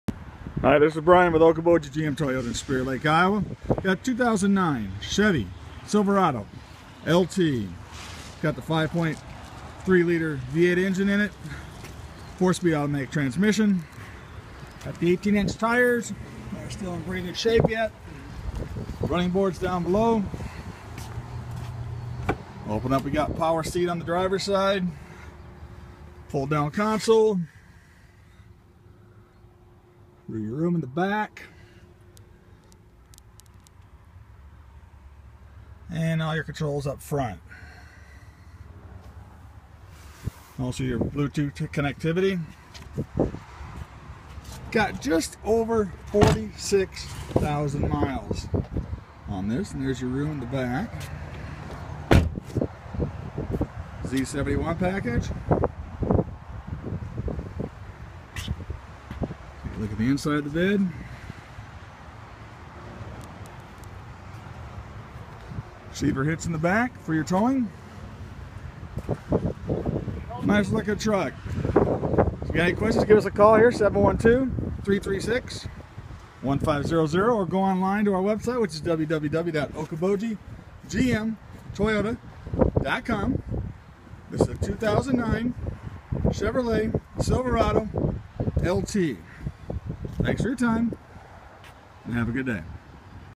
2009 CHEVROLET SILVERADO LT OKOBOJI MOTOR COMPANY SPIRIT LAKE, IOWA
The 2009 Chevrolet Silverado LT is for sale at Okoboji Motor Company in Spirit Lake, Iowa.